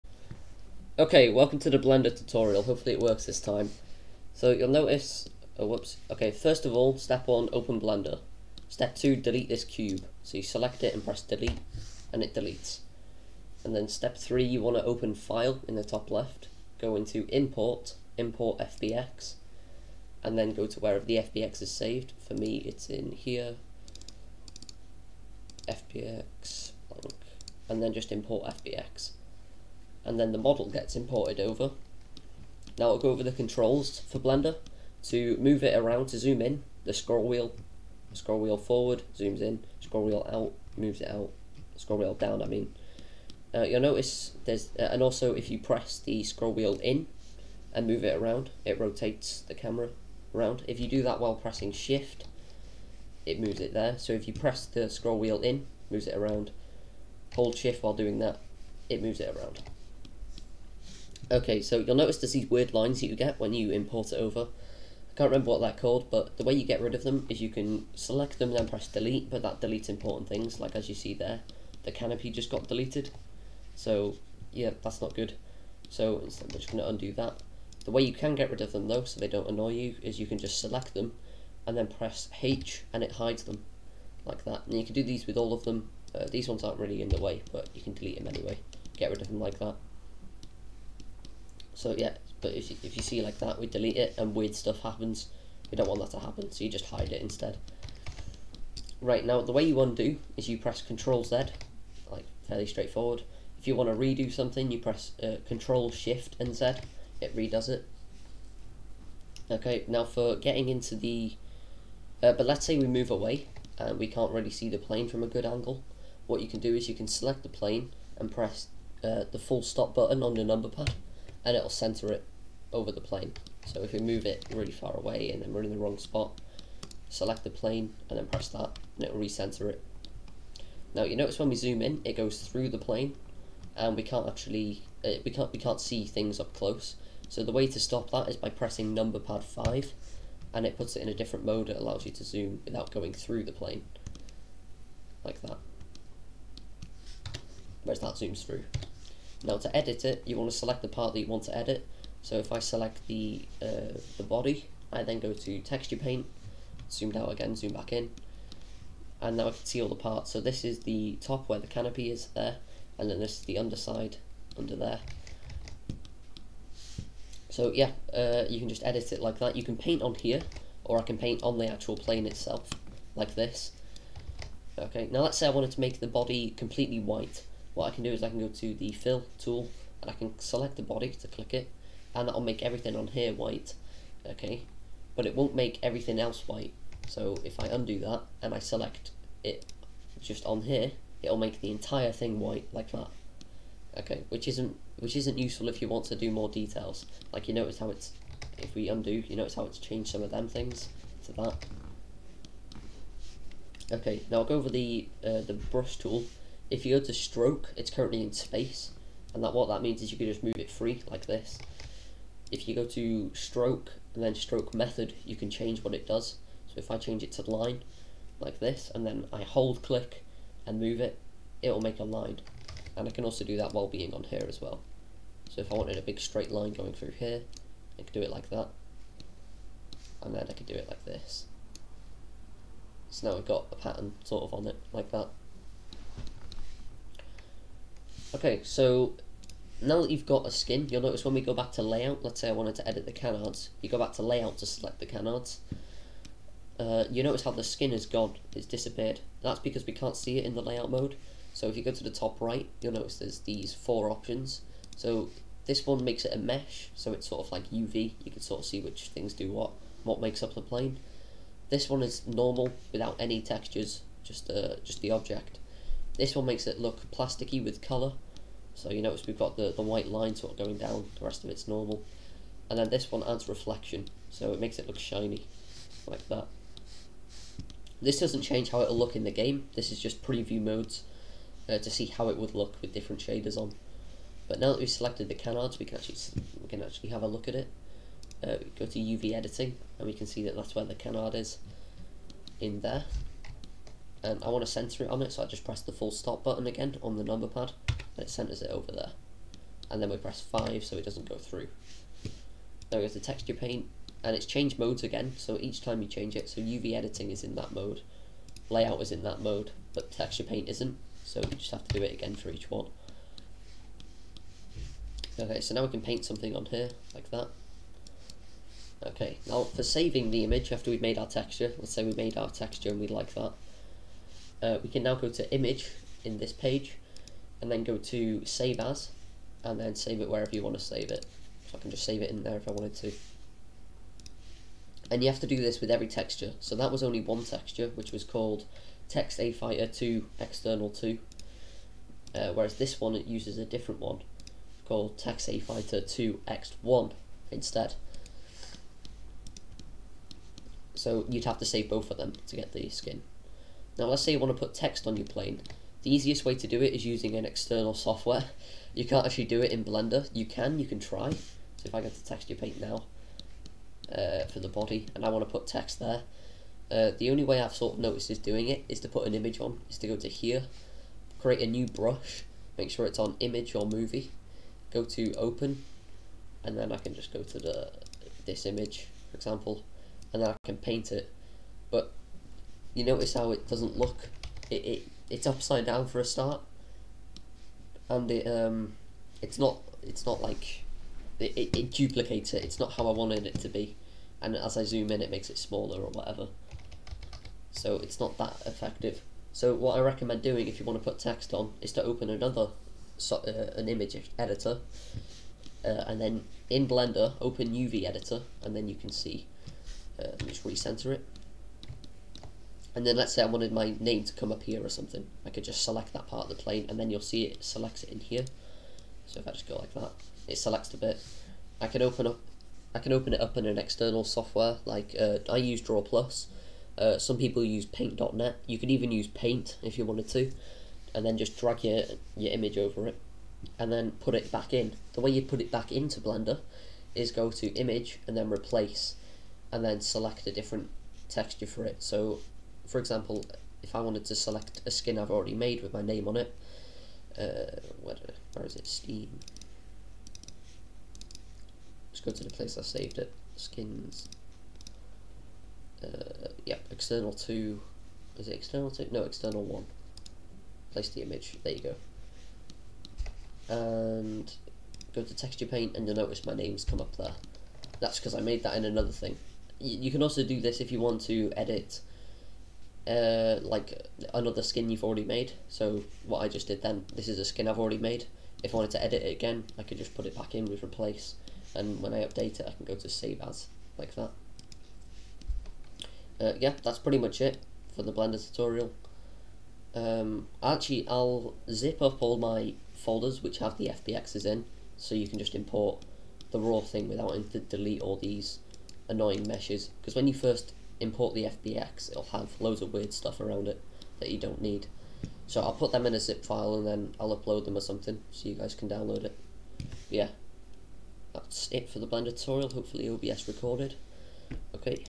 Blender Tutorial For VTOL
This took 6 attempts because OBS screwed up, please excuse the dying voice.

Keybinds:
Undo - Ctrl + Z
Redo - Crtrl + Shift + Z
Zoom in/out - Scroll up/down
Rotate camera - Press scroll wheel down
Pan camera - Press scroll wheel down + Shift
Center camera on selected part - NumFullstop
Toggle zoom mode - NUM5